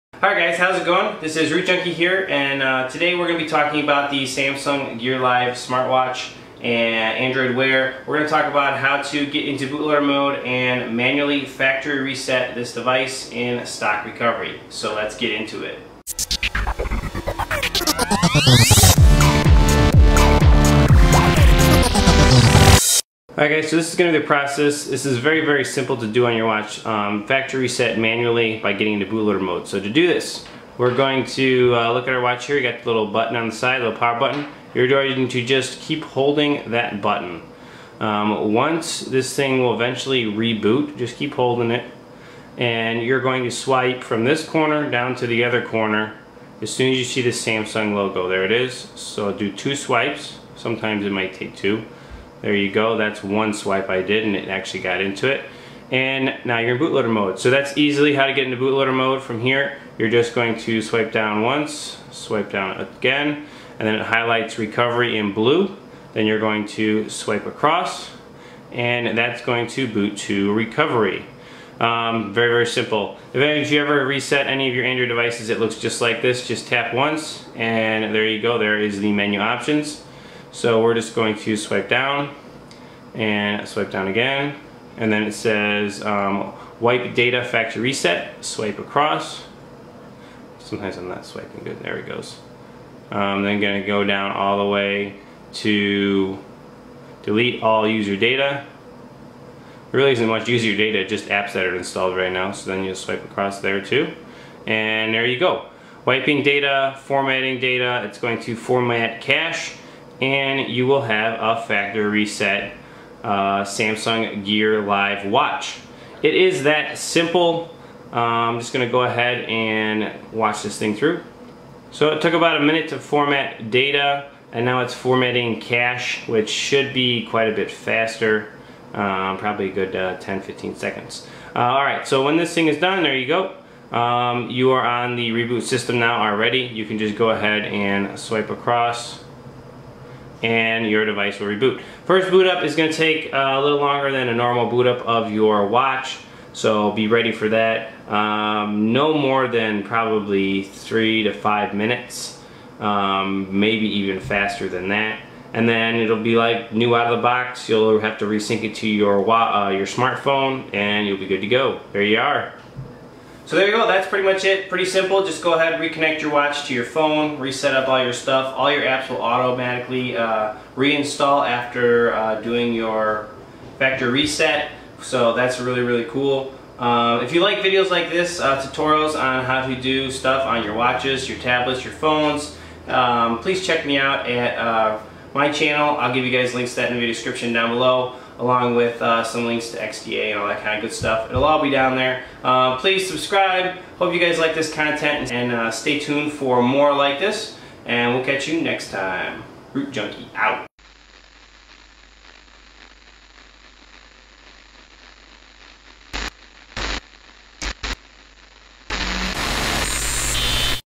How to Factory Reset your Samsung Gear Live Android Wear SmartWatch
Do you want to Factory Reset your Samsung Gear Live Smartwatch? This can be either because you had a bad flash, you installed an app and now things just aren't right, or because it's just slowing down in general. There are many reasons to factory reset your device. The steps are quick and easy, but they are unique. The button combinations are different than most other devices because there really is only one button.

In today's video, XDA Developer TV newcomer and XDA Recognized Contributor RootJunky shows off how to manually boot into the Samsung Gear Live's bootloaders and factory reset the device. Grab your device and check out this video.